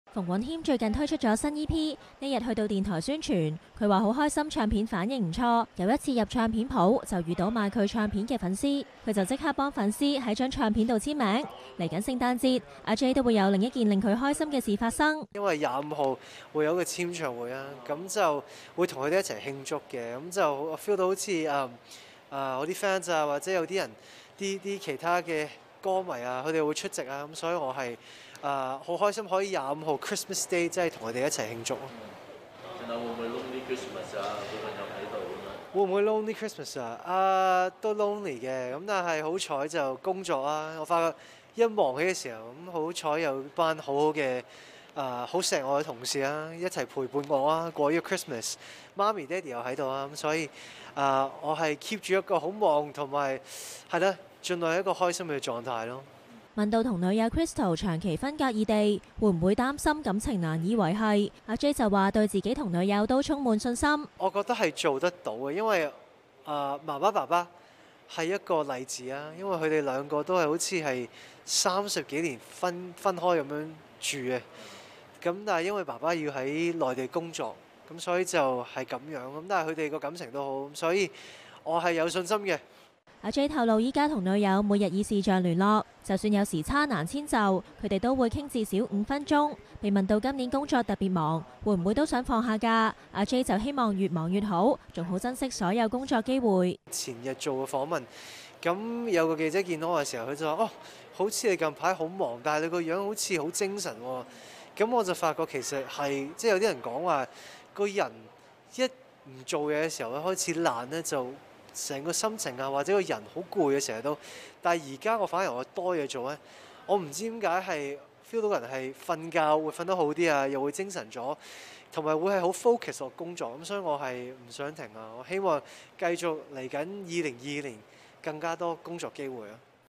馮允謙聖誕舉行簽唱會 - 20211224 - 娛樂頭條E-news Headline
馮允謙最近推出新EP，這天他去到電台宣傳，他很開心唱片反應不錯，有一次進唱片店，就遇到買他唱片的粉絲，他就即時幫粉絲在唱片上簽名，聖誕節將至，Jay都會有另一件令他開心的事發生，就是25號有個簽唱會，會跟粉絲一起慶祝。

相關影片:
1. 何詩蓓破世界紀錄片段重温！世錦賽 1 分 50 秒 31奪金！－有線電視